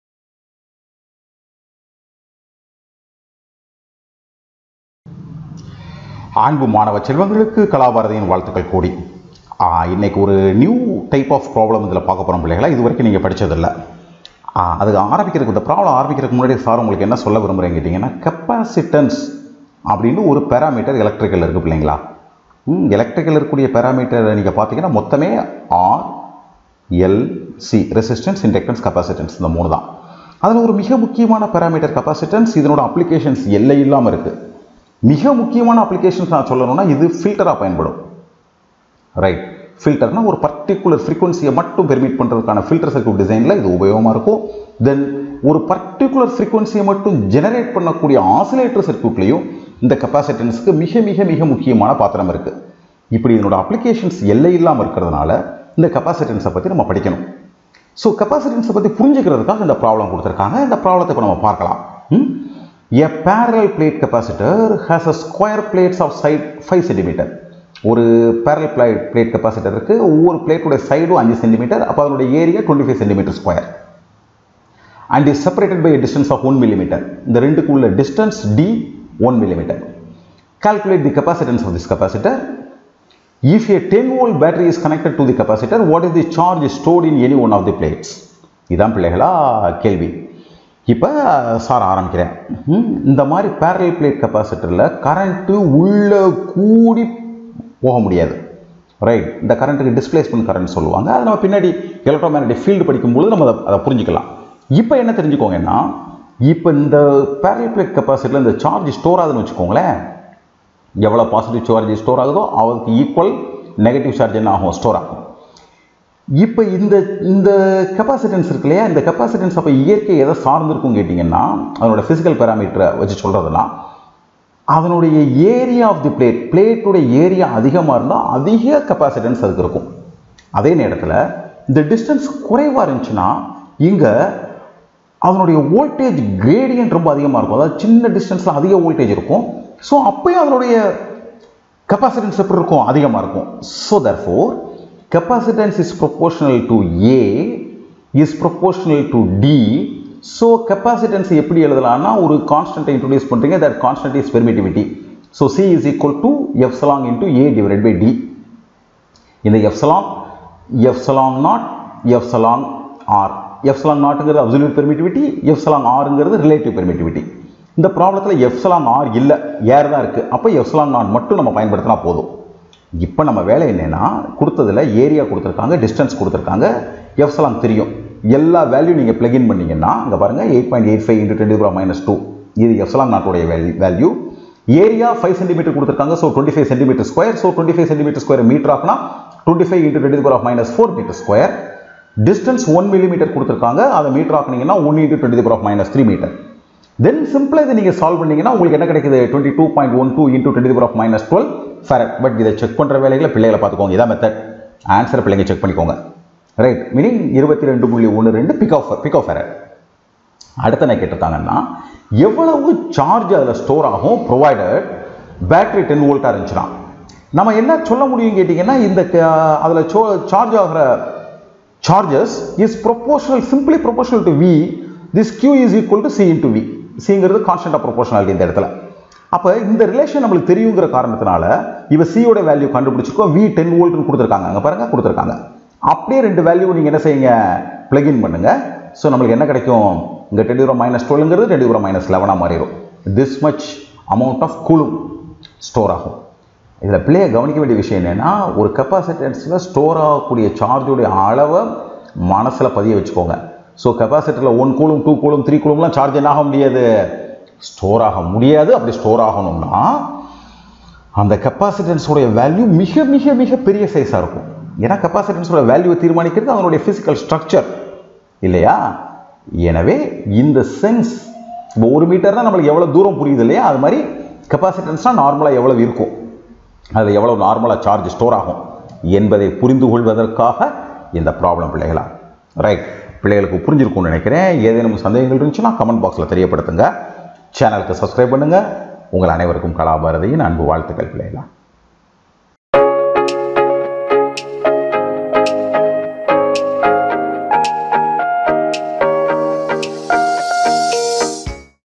20.Physics | Electrostatics | Example 1.20
A parallel plate capacitor has square plates of side 5 cm and separated by a distance of 1 mm. (a) Calculate the capacitance of this capacitor. (b) If a 10 V battery is connected to the capacitor, what is the charge stored in any one of the plates?
(The value of εo = 8.85 x 10-12 Nm2 C-2)